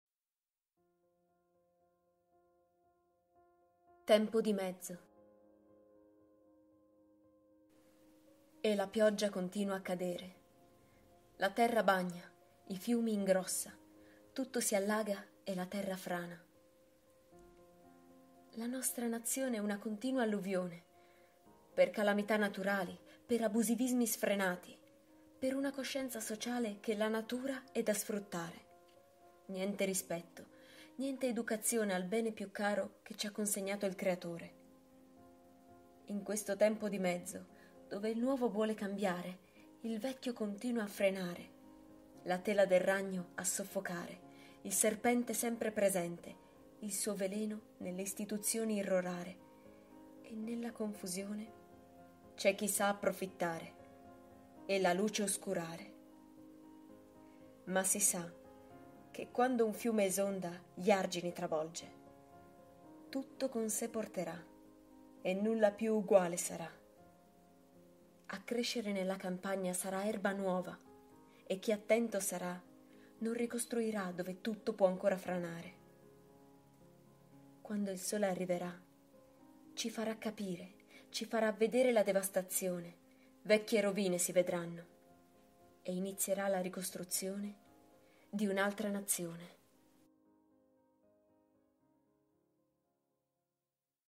Mery Brescianini 13 Tempo di mezzo
«Sono nata sul Lago d’Iseo ma quand’ero piccola i miei si sono tra- sferiti in Brianza, dove tuttora vivo; alla fine del periodo lavorativo ho coltivato la mia passione di sempre: scrivere poesie, l’anno scor- so ho pubblicato attraverso Albatros una silloge intitolata Le mie prime 50 poesie vere e di fantasia».